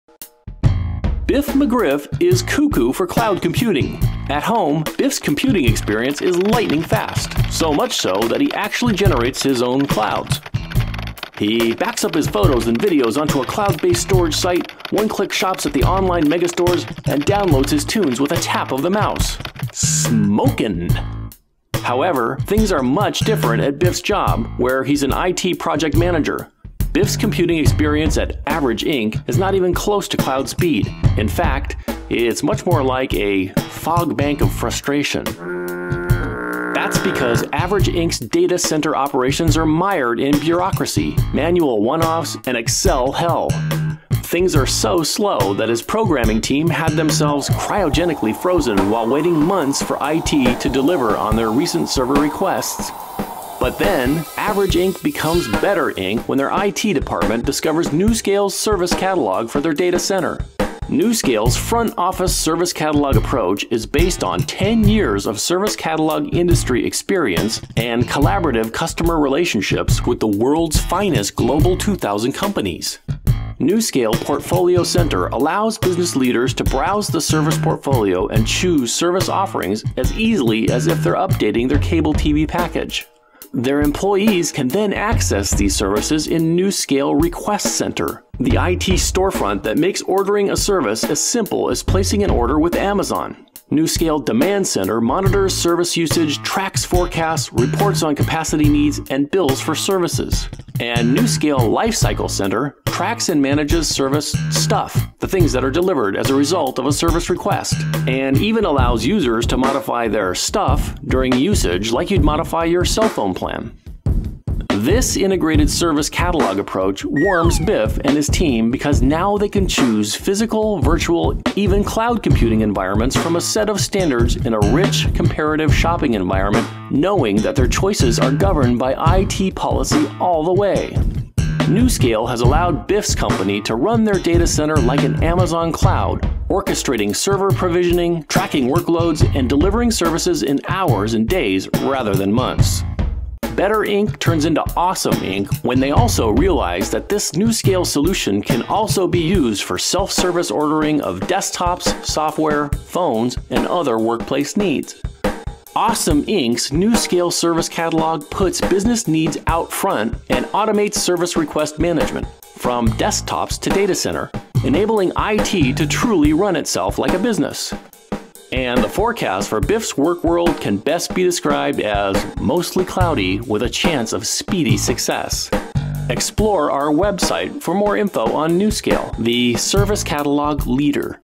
newScale 3-Minute Explainer Video.mp4
Three-minute animated video that explains how newScale's market-leading Service Catalog solutions create a self-service IT storefront that drives standards and accelerates business success - from desktop to data center.